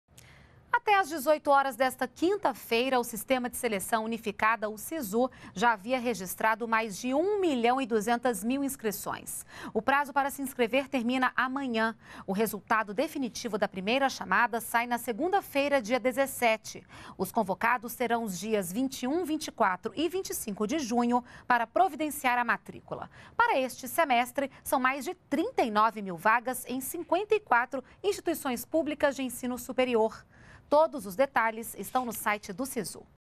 Mais de 600 mil pessoas já se inscreveram no Sisu
NBR NOTÍCIAS - 13.06.13: Até às 18 horas desta quinta-feira (13), o Sistema de Seleção Unificada (Sisu) já havia registrado cerca de 1,2 milhão de inscrições, com mais de  640 mil candidatos. O prazo termina nesta sexta-feira. O resultado definitivo da primeira chamada sai na segunda-feira, dia 17. Os convocados terão os dias 21, 24 e 25 de junho para providenciar a matrícula. Para este semestre são mais de 39 mil vagas em 54 instituições públicas de ensino superior. Mais informações: sisu.mec.gov.br